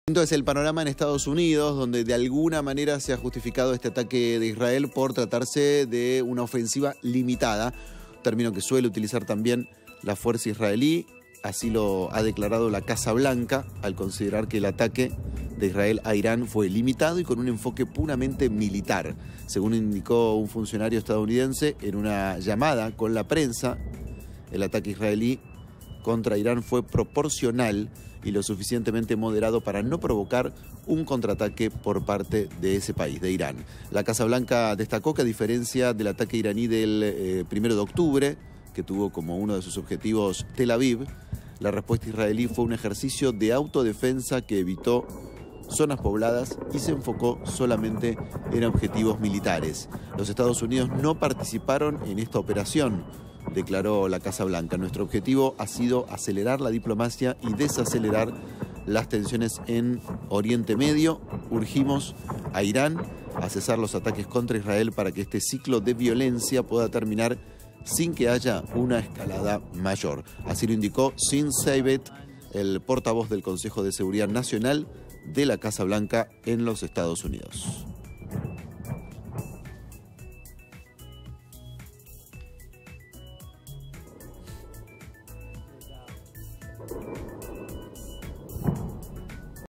EEUU resaltó la proporcionalidad del ataque contra Irán
👉 La Casa Blanca consideró que el ataque israelí a Irán ha sido limitado y con un enfoque puramente militar. Según indicó un funcionario estadounidense en una llamada con la prensa, el ataque israelí de esta noche contra Irán fue "proporcional" y lo suficientemente moderado para no provocar un contrataque por parte de Irán.